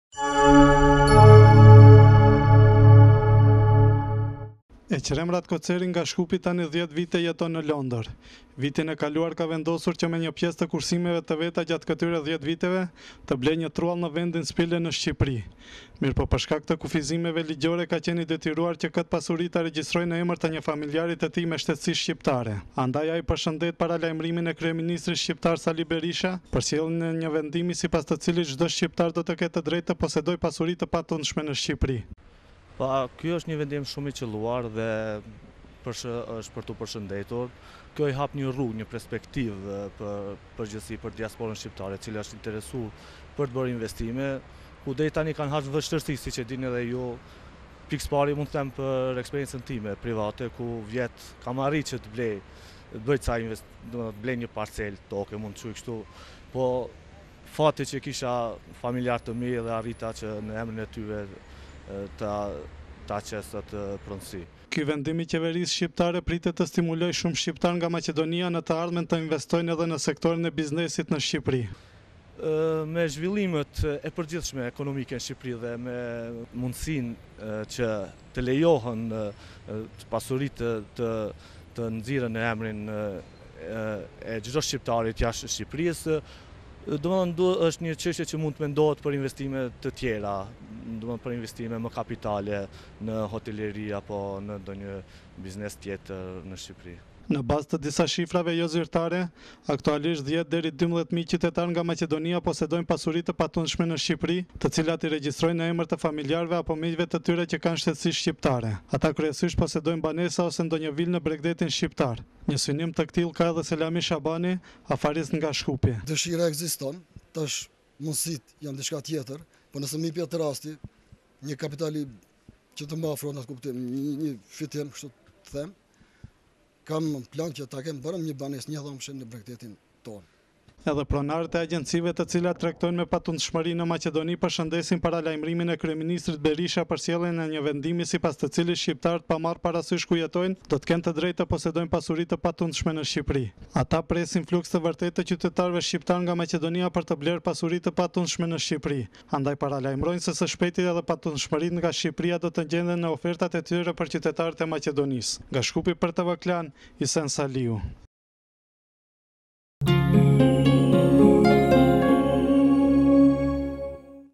Vendimi i Berishës shtyn shqiptarët e Maqedonisë të blejnë toka në Shqipëri-(9 Gusht 2009)
Eqrem Ratkoceri nga Shkupi tani dhjetë vite jeton në Londër. Vitin e kaluar ka vendosur që me një pjesë të kursimeve të veta gjatë këtyre dhjetë viteve të blejë një truall në vendin Spille në Shqipëri.

Mirëpo për shkak të kufizimeve ligjore ka qenë i detyruar që këtë pasuri ta regjistrojë në emër të një familjari të tij me shtetësi shqiptare, andaj ai përshëndet paralajmërimin e kryeministrit shqiptar, Sali Berisha, për sjelljen e një vendimi sipas të cilit çdo shqiptar do të ketë të drejtë të posedojë pasuri të patundshme në Shqipëri.

Eqrem Ratkoceri: “Ky është një vendim shumë i qëlluar dhe është për t’u përshëndetur. Kjo i hap një rrugë një perspektive, në përgjithësi për diasporën shqiptare, e cila është interesu për të bërë investime, ku deri tani kanë has vështirësi, siç e dini dhe ju. Pikë së pari mund të them për eksperiencën time private, ku vjet kam arrit që të blej një parcelë toke, mund të quaj kështu. Po fati që kisha familjarët e mi dhe arrita që në emrin e tyre ta qes atë pronësi”.

Ky vendim i qeverisë shqiptare pritet të stimulojë shumë shqiptarë nga Maqedonia në të ardhmen të investojnë edhe në sektorin e biznesit në Shqipëri.

Eqrem Ratkoceri: “Me zhvillimet e përgjithshme ekonomike në Shqipëri dhe me mundësinë që të lejohen pasuritë të nxirren në emrin e çdo shqiptari jashtë Shqipërisë, dmth, është një çështje që mund të mendohet për investime të tjera, dmth për investime më kapitale në hoteleri apo në ndonjë biznes tjetër në Shqipëri”.

Në bazë të disa shifrave jozyrtare aktualisht 10 deri 12 mijë qytetarë nga Maqedonia posedojnë pasuri të patundshme në Shqipëri, të cilat i regjistrojnë në emër të familjarëve apo miqve të tyre që kanë shtetësi shqiptare. Ata kryesisht posedojnë banesa ose ndonjë vilë në bregdetin shqiptar. Një synim të këtillë ka edhe Selami Shabani, afarist nga Shkupi.

Selami Shabani: “Dëshira ekziston, tash mundësitë janë diçka tjetër. Po nëse më jepet rasti një kapitali që të më afrohet në atë kuptim, një fitim kështu të them, kam në plan që ta kem një banesë 1 dhomëshe në bregdetin tonë”.

Edhe pronarët e agjencive, të cilat tregtojnë me patundshmëri në Maqedoni përshëndesin paralajmërimin e kryeministrit Berisha për sjelljen e një vendimi sipas të cilit shqiptarët pa marrë parasysh ku jetojnë do të kenë të drejtë të posedojnë pasuri të patundshme në Shqipëri.

Ata presin fluks të vërtetë të qytetarëve shqiptarë nga Maqedonia për të blerë patundshmëri në Shqipëri, andaj paralajmërojnë se së shpejti edhe patundshmëritë nga Shqipëri do të gjenden në ofertat e tyre për qytetarët e Maqedonisë. Nga Shkupi për TV Klan, Isen Saliu./tvklan.al